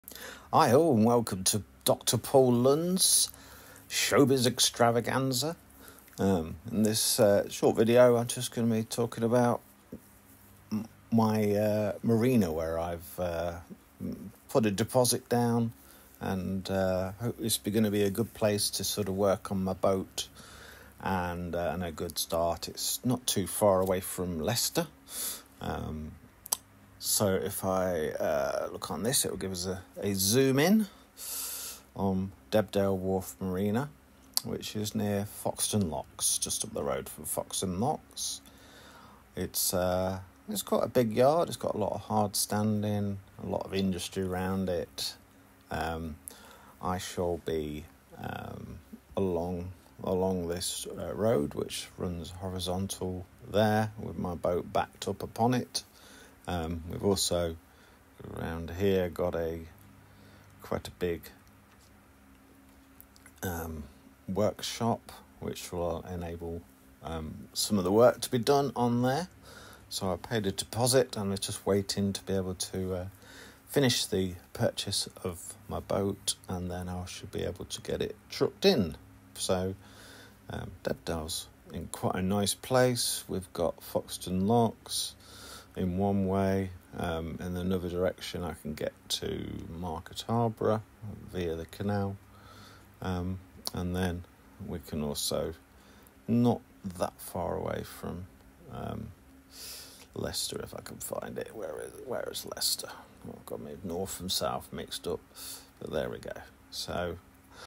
4 Debdale Wharf Marina
Short video showing where i will be moored in Leicestershire - at Debdale Wharf marina near Foxton Locks